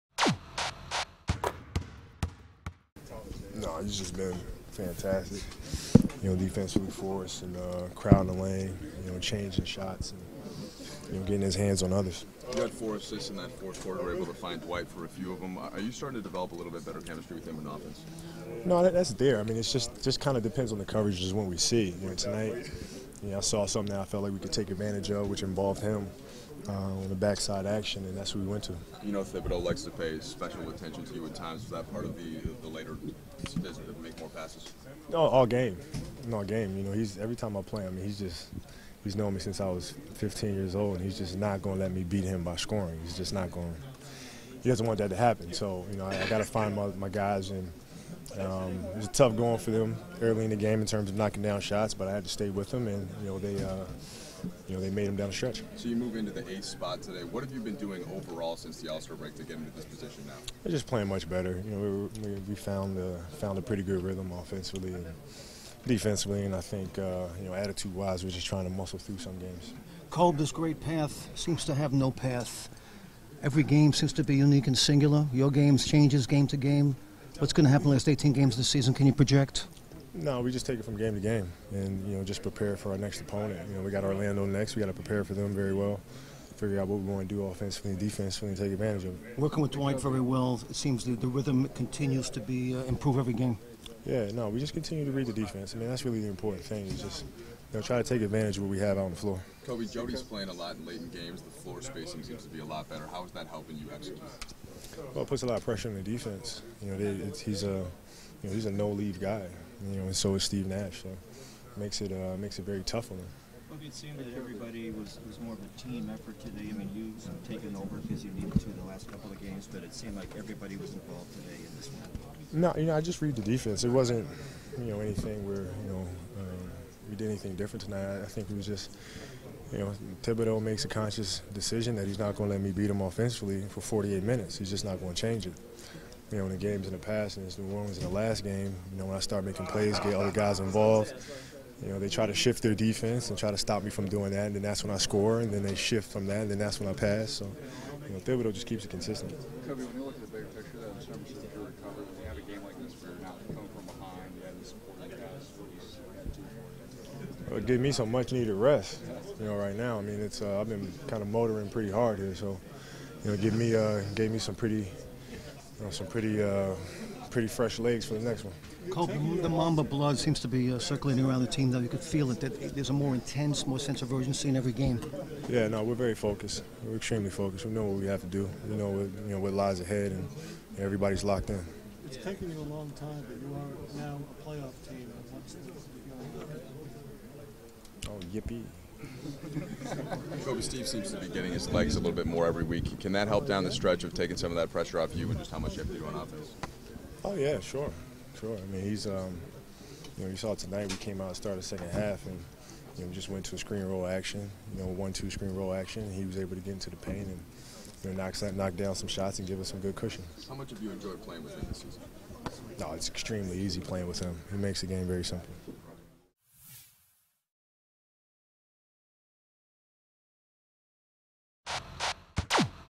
Postgame: Kobe Bryant | Lakers Vs Bulls | March 10, 2013
Dwight Howard scores 16 points and pulls down 21 rebounds to help lift the Lakers past the Bulls, 90-81.